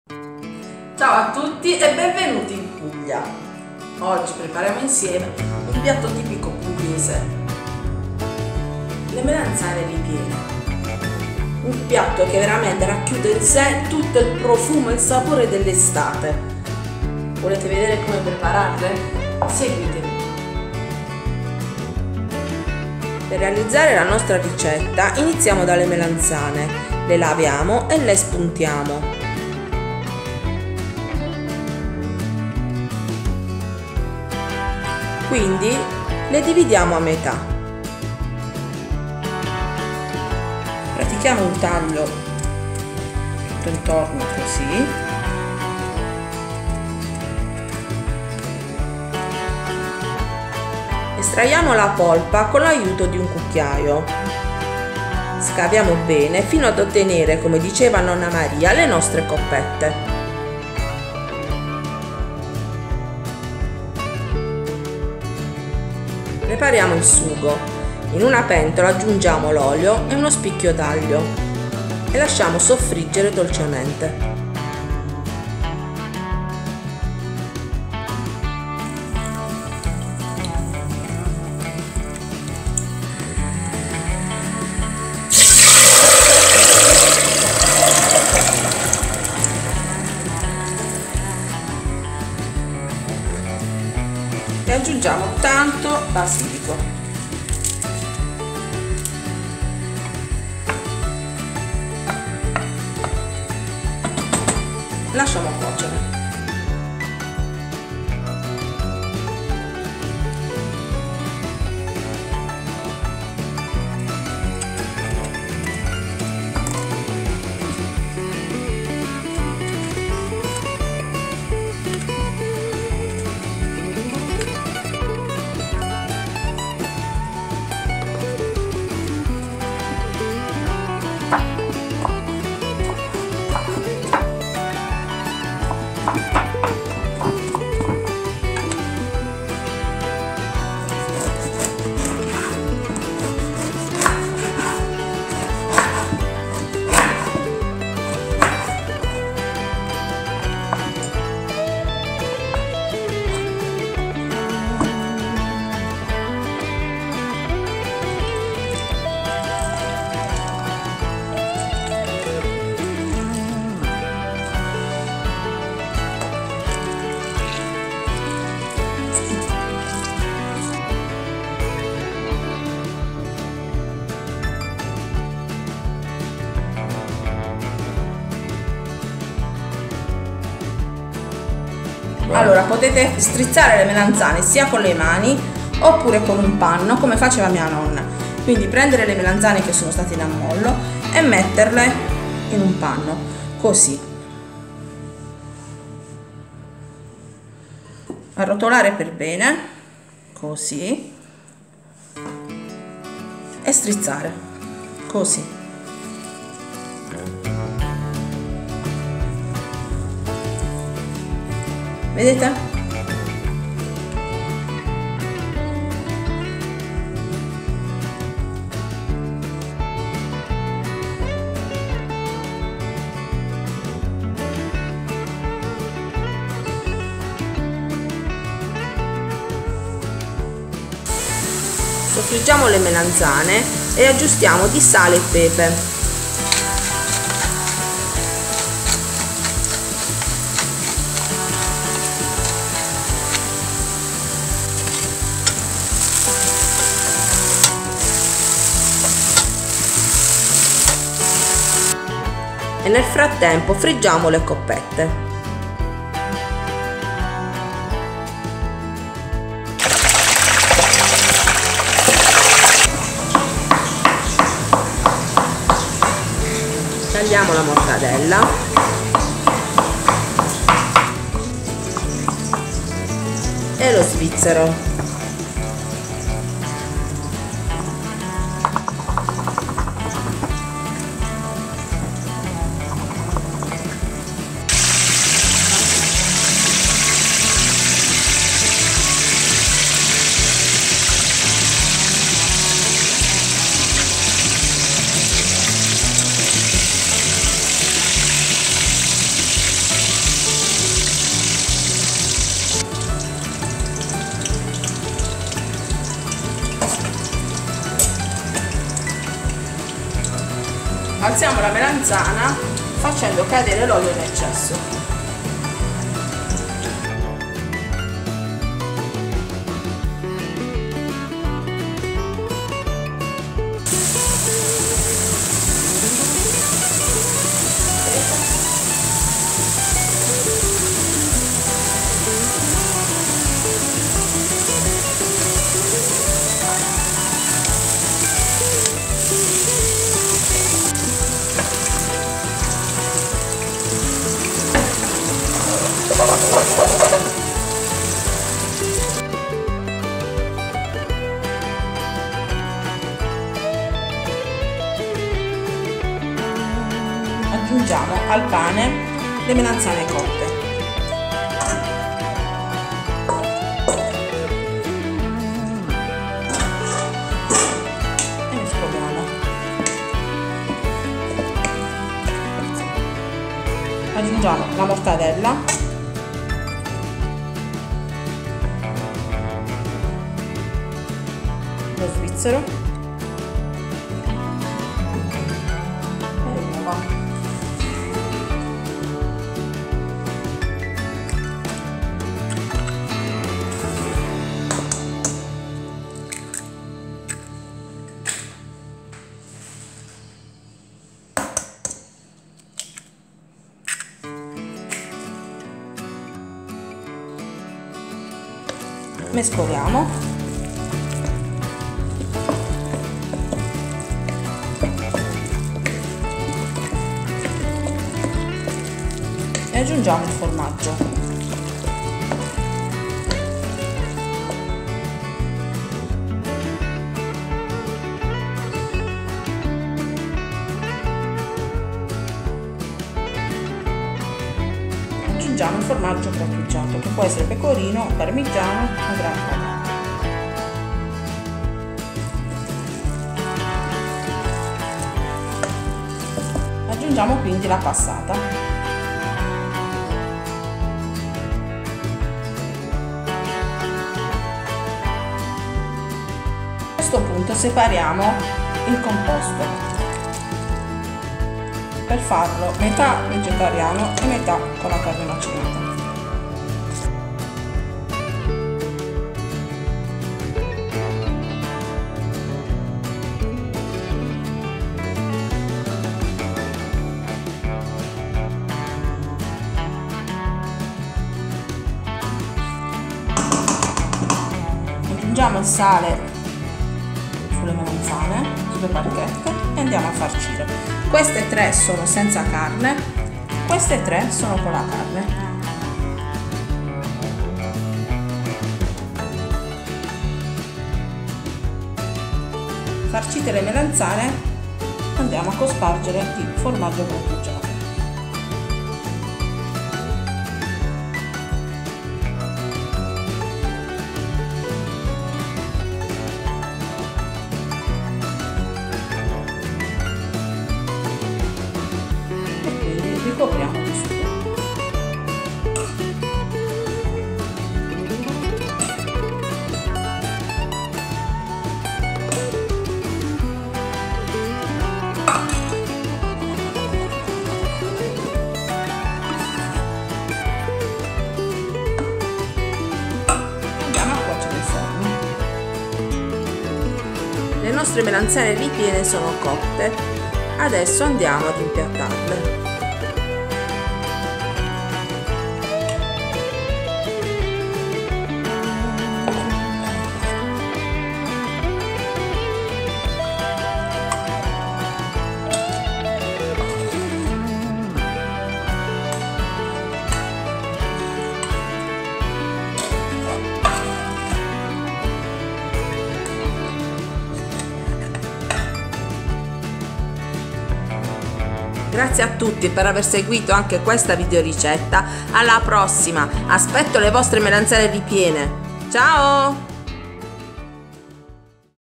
MELANZANE RIPIENE - Ricetta di cucina con me Graziella
MELANZANE RIPIENE - RICETTA PASSO PASSO

Le Ricette di CUCINA CON ME GRAZIELLA

Le Melanzane ripiene, un  piatto tipico della mia terra la Puglia,  sono un must nelle serate estive da gustare in riva al mare accompagnate da una buona focaccia.
Questa ricetta è un secondo piatto , in realtà si tratta un piatto unico , un piatto che racchiude in se tutti i profumi e i sapori dell’ estate : le melanzane di stagione , i pomodori belli rossi e maturi , il basilico.

Prepararle  è un atto d’ amore grande per la propria famiglia vista la lunga lavorazione e i vari passaggi che questa ricetta prevede , amore che  passando dal cuore alle mani diventa magia.

Ed è così che nelle calde sere estive ,tradizione vuole che ci si incontri in riva al mare con famiglia e parenti al seguito a mangiare le melanzane ripiene rigorosamente accompagnate da focaccia e caroselli , con le teglie sistemate su tavoli di fortuna e tanta allegria il tutto condito da  del buon vino rosso.

Le melanzane ripiene , quindi , diventano un simbolo di unione una “scusa ” per stare insieme.

Come per tutte le ricette della tradizione,  anche per le melanzane ripiene,  esistono diverse versioni , che cambiano di casa in casa , ricette tutte buone a volte,  però,  profondamente diverse.

La ricetta di cui sto per parlarvi è quella di di nonna Maria e delle sue melanzane ripiene che realizzava in due diverse versioni : una vegetariana e una con il macinato di carne .

                                 LE  MELANZANE RIPIENE

★  SEGUIMI   SUI SOCIAL       𝗙𝗢𝗟𝗟𝗢𝗪 𝗠𝗘  SOCIAL  ★

■ 𝗖𝗼𝗻𝘁𝗮𝘁𝘁𝗶